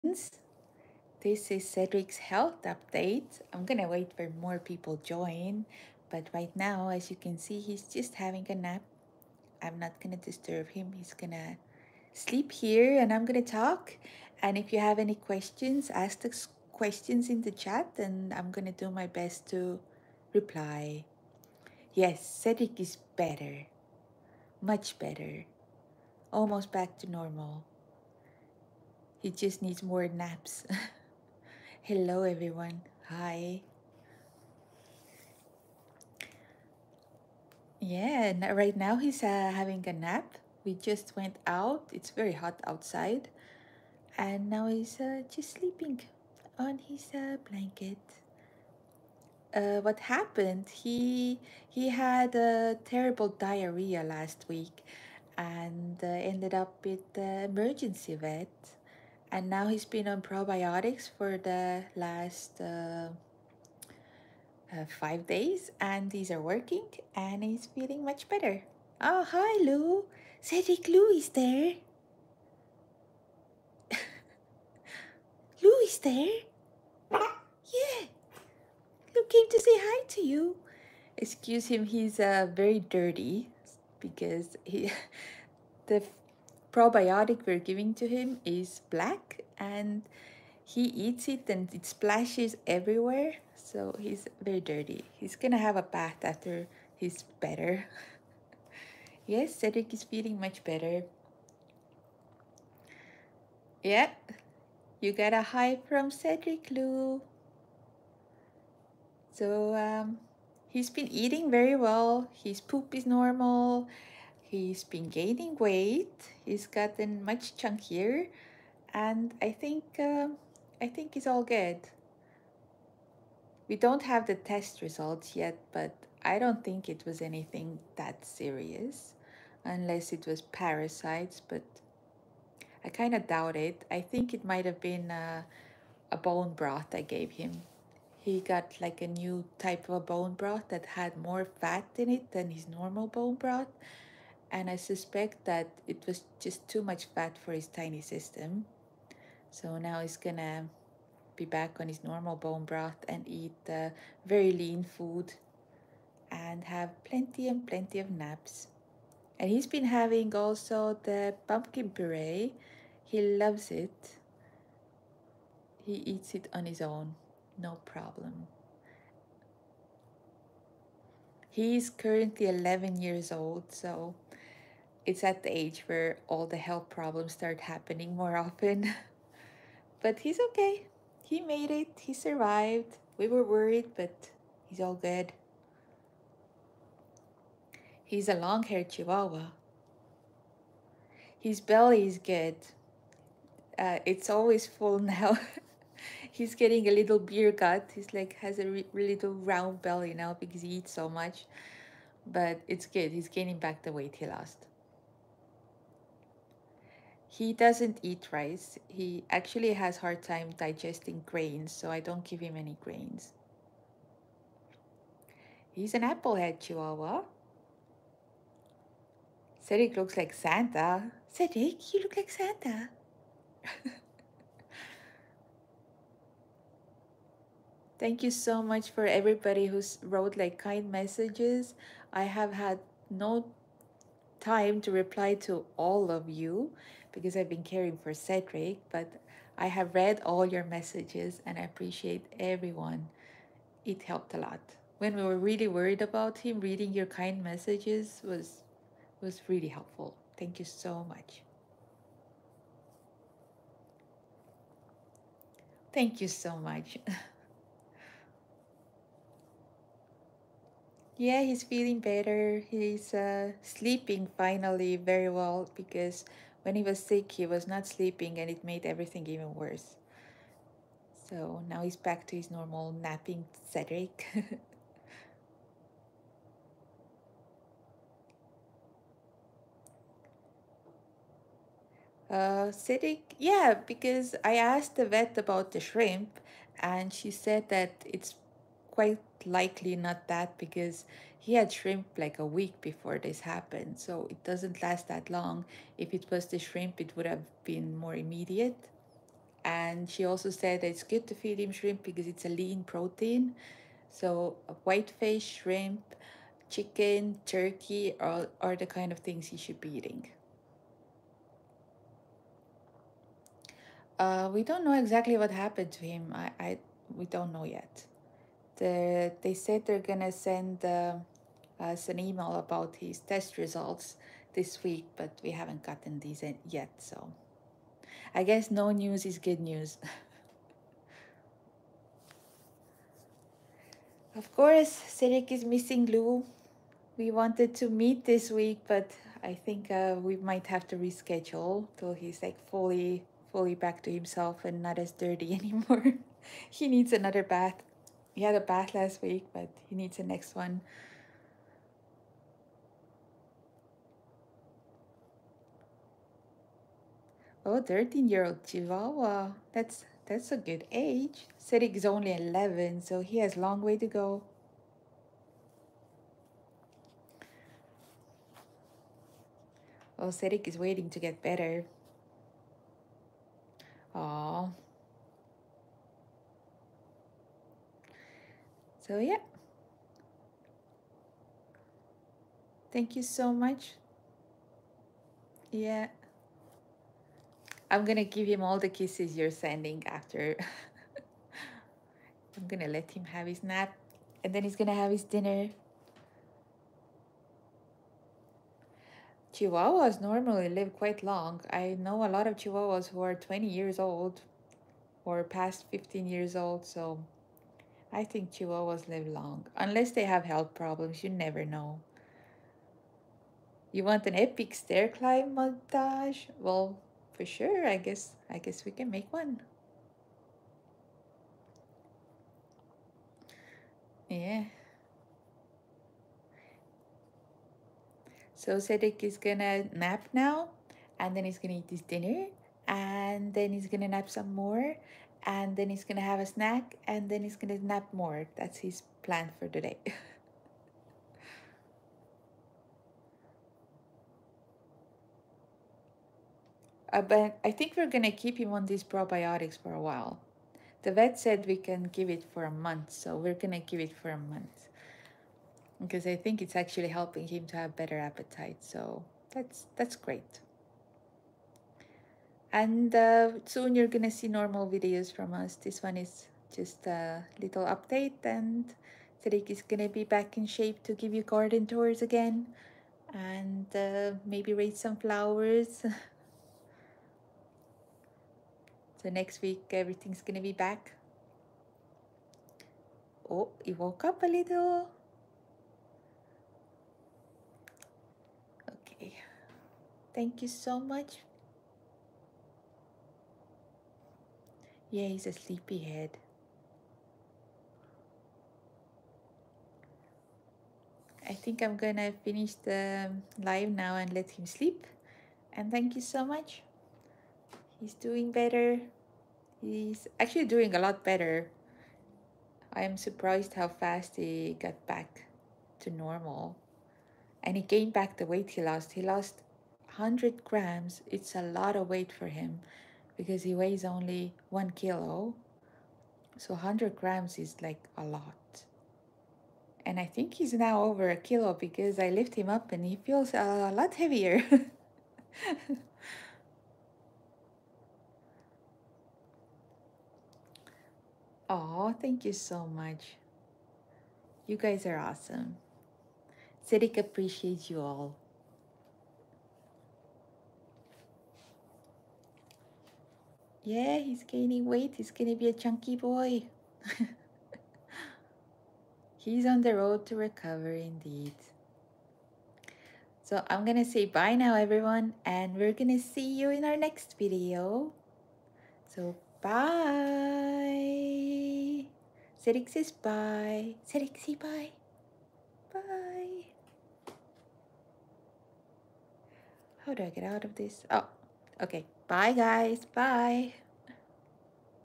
Cedric’s health update! (live)
Cedric had a health scare. We ended up at the emergency vet late at night and spent many hours there. He's a very tiny chihuahua and even the smallest health problem can turn into a serious issue very fast. It was very stressful and scary for everyone. He had been sick over a day and not eating or keeping food in. When his situation was not getting better at all, but only worse over time, we rushed to the animal hospital's emergency to get him some help!
This is his health update!

Chihuahua Cedric
Brossard QC
J4Z 3R6
CANADA